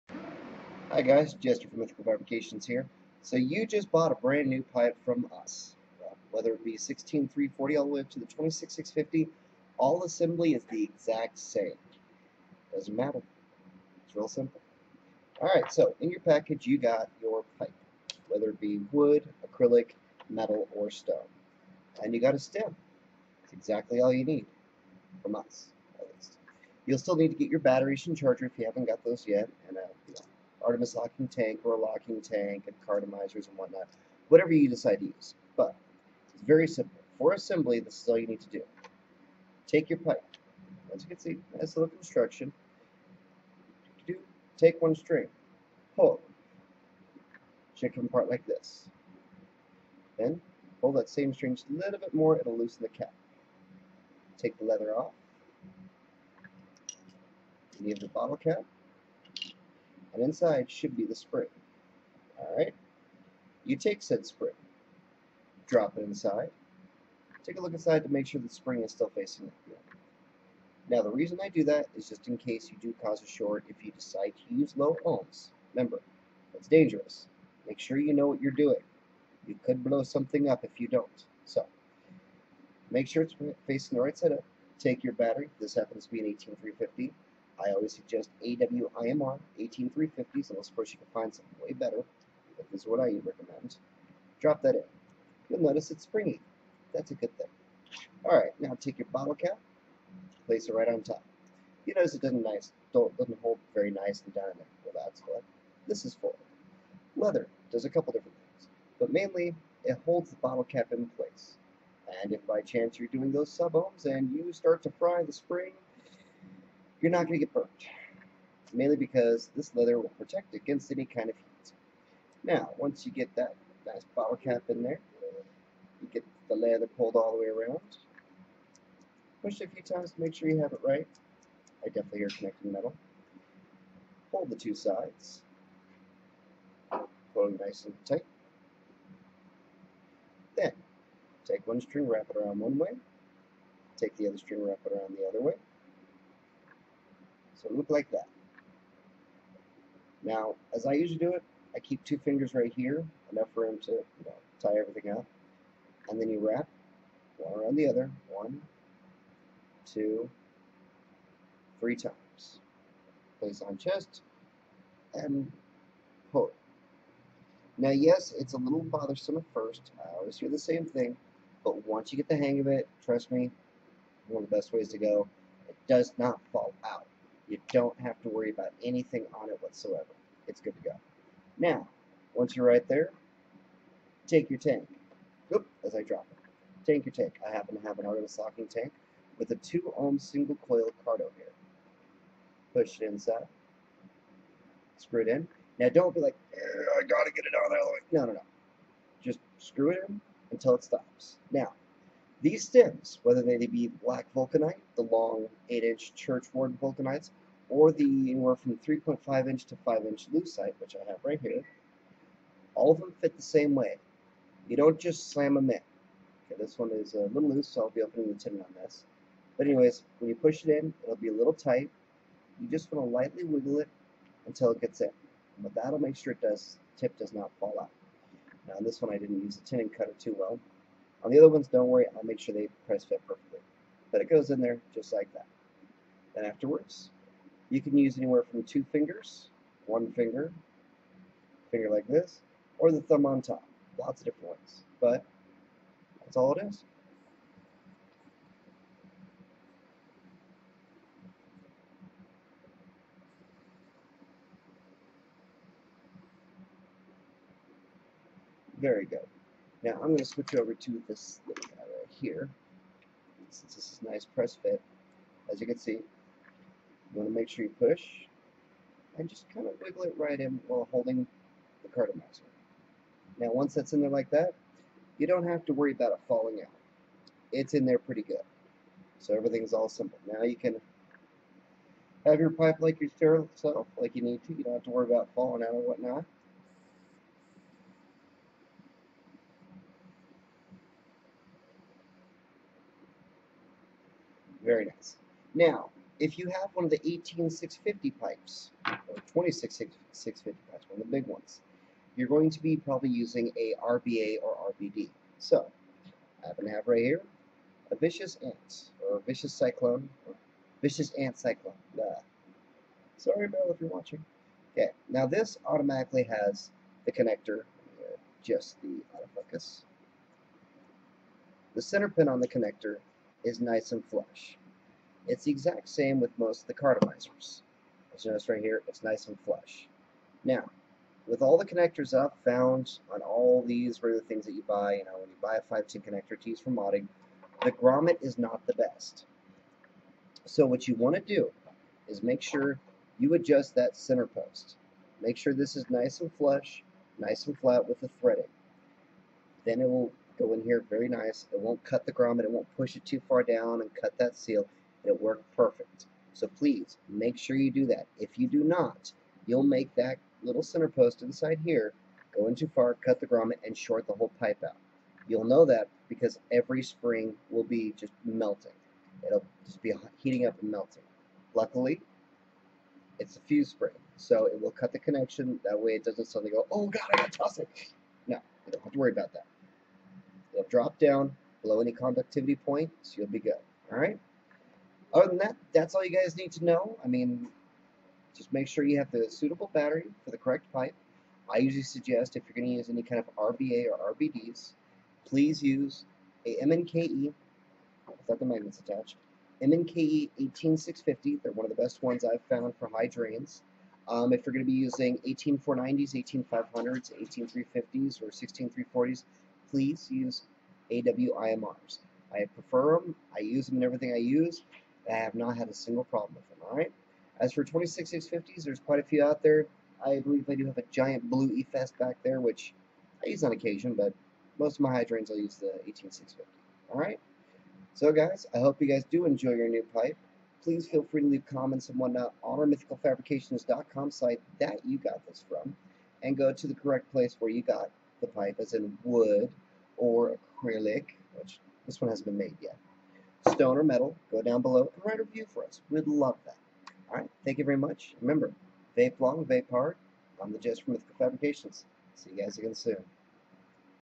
Instructions on your new Mythical Fabrications Epipe
This is how you put it together and get it to work.

Our Spiffy pretty site:
MythicalFabrications.com